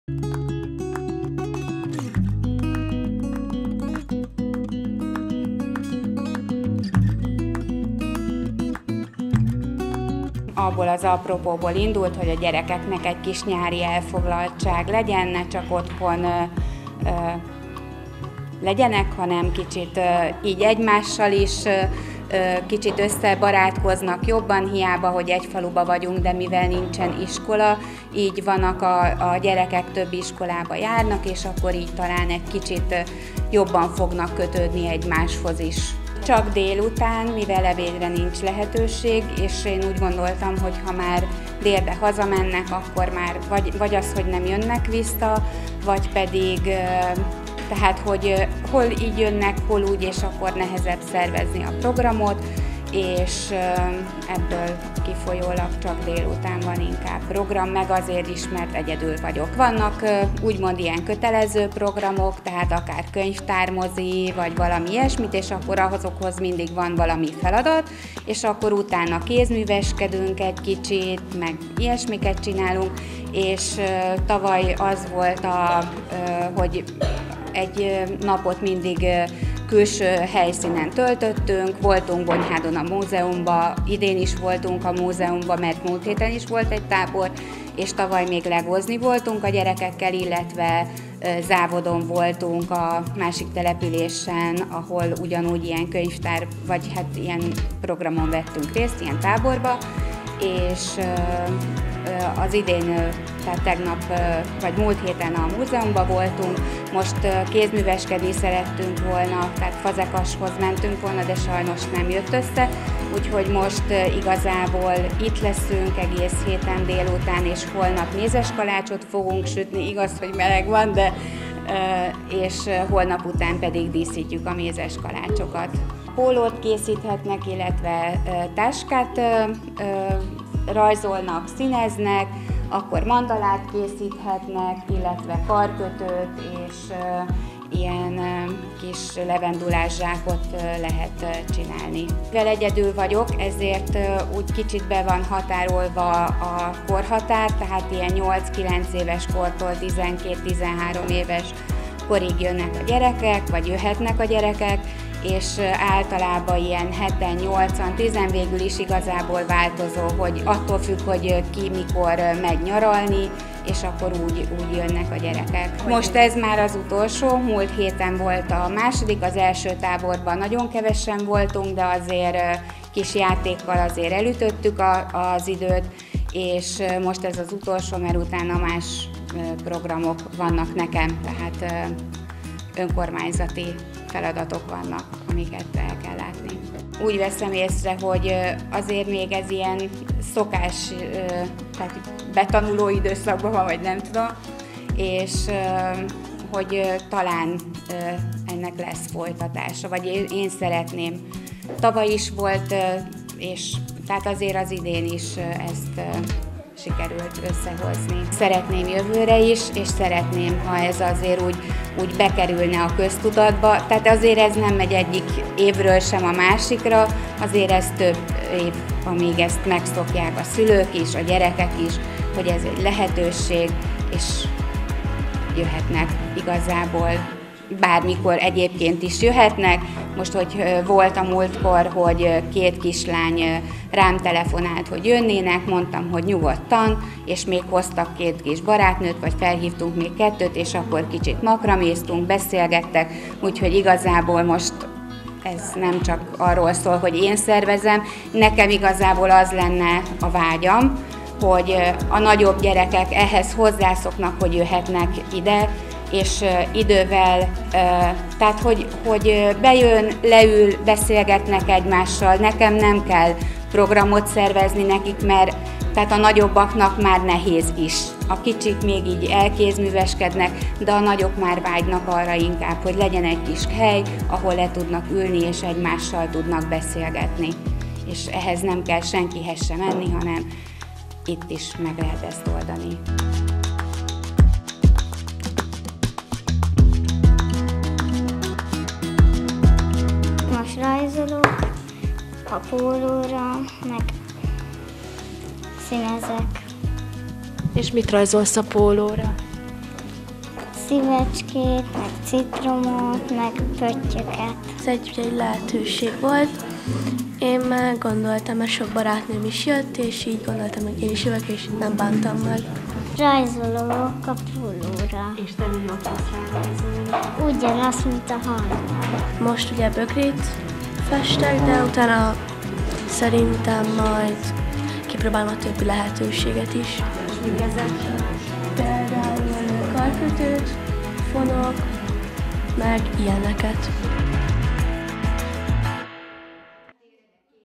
Napközis tábor - a kisdorogi művelődési házben kézműveskedtek a gyerekek
A tavalyi évhez hasonlóan idén is szerveztek napközis táborban a kisdorogi iskolásoknak a helyi művelődési házban. A nyári szünetben a gyerekeknek kézműves foglalkozással készültek, ezenfelül filmvetítés is várta őket.